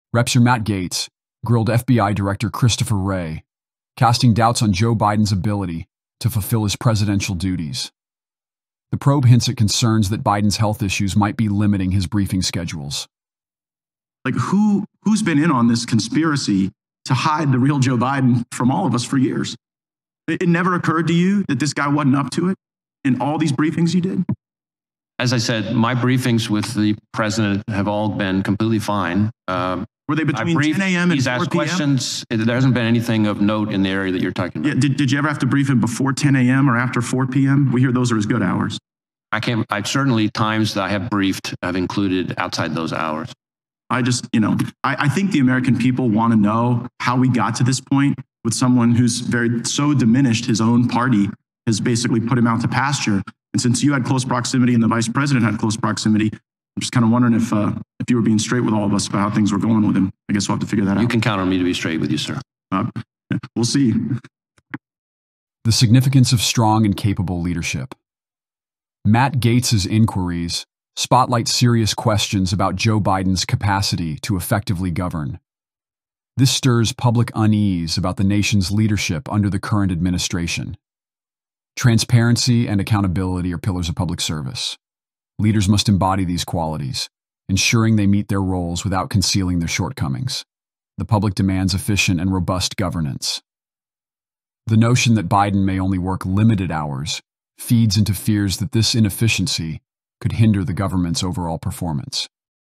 Rep. Matt Gaetz Grills FBI Director Wray on Biden's Briefing Hours
Rep. Matt Gaetz to FBI Director Wray: Biden's Secret Hours Exposed! In a gripping exchange, Rep. Matt Gaetz confronts FBI Director Christopher Wray about President Biden's unusual briefing hours. Are there hidden aspects of Biden's schedule that the American public should know about? Gaetz raises questions about the President's fitness and the transparency of his administration.